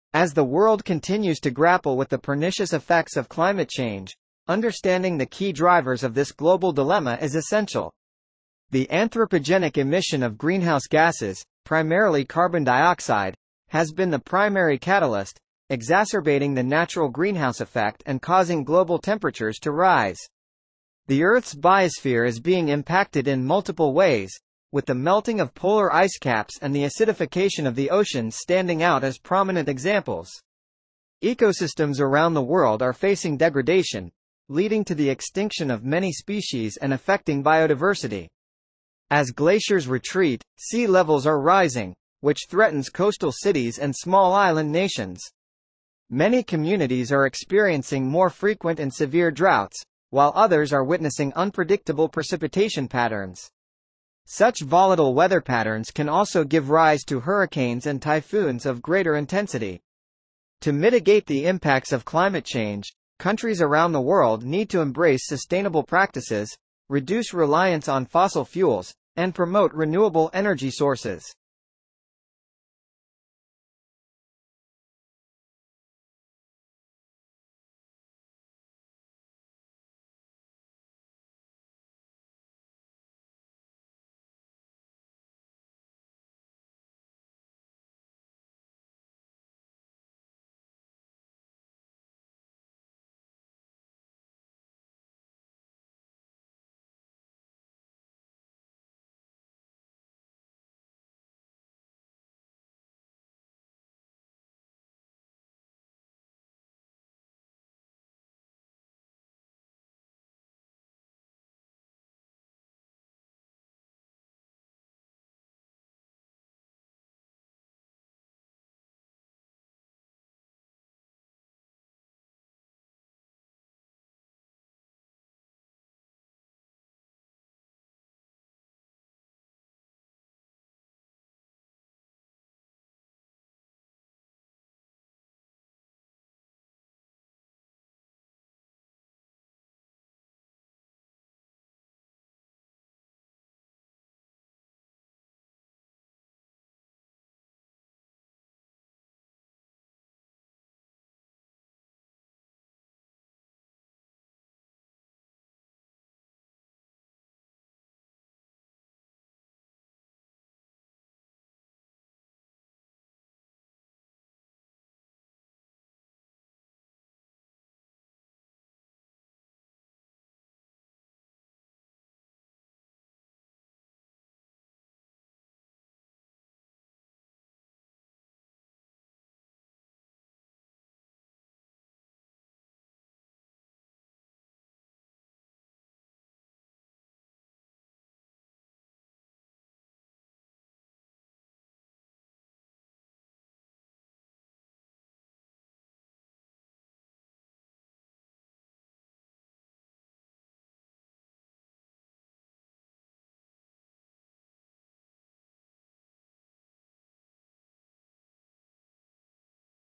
Climate Change : For English learners—Listen and read the passage in the description below.
A Reading Passage on “Climate Change”

As the world continues to grapple with the pernicious effects of climate change, understanding the key drivers of this global dilemma is essential. The anthropogenic emission of greenhouse gases, primarily carbon dioxide, has been the primary catalyst, exacerbating the natural greenhouse effect and causing global temperatures to rise. The Earth's biosphere is being impacted in multiple ways, with the melting of polar ice caps and the acidification of the oceans standing out as prominent examples. Ecosystems around the world are facing degradation, leading to the extinction of many species and affecting biodiversity. As glaciers retreat, sea levels are rising, which threatens coastal cities and small island nations. Many communities are experiencing more frequent and severe droughts, while others are witnessing unpredictable precipitation patterns. Such volatile weather patterns can also give rise to hurricanes and typhoons of greater intensity. To mitigate the impacts of climate change, countries around the world need to embrace sustainable practices, reduce reliance on fossil fuels, and promote renewable energy sources.

List of Difficult Words with Definitions (in context):

Anthropogenic: Caused or produced by humans.
Exacerbating: Making a situation worse or more severe.
Biosphere: The regions of the surface and atmosphere of the Earth where living organisms exist.
Melting: Becoming liquefied from heat (in this context, it refers to ice turning into water).
Polar ice caps: Thick ice sheets that cover the North and South Poles of the Earth.
Acidification: The process by which natural waters become more acidic due to certain pollutants.
Ecosystems: A community of living organisms and their physical environment.
Degradation: The process by which something is damaged or worsened.
Extinction: The state in which a species no longer exists.
Glaciers: A slowly moving mass of ice formed by the accumulation and compaction of snow.
Droughts: Prolonged periods of abnormally low rainfall, leading to a shortage of water.
Precipitation: The action or process of precipitating a substance from a solution, often referring to rain, snow, or hail.
Volatile: Liable to change rapidly and unpredictably.
Hurricanes: A storm with a violent wind, specifically in the Atlantic Ocean.
Typhoons: A tropical storm in the region of the Indian or western Pacific oceans, similar to a hurricane but in a different geographic region.
Mitigate: To make something less severe, serious, or painful.
Renewable energy: Energy from a source that is not depleted when used, like wind or solar power.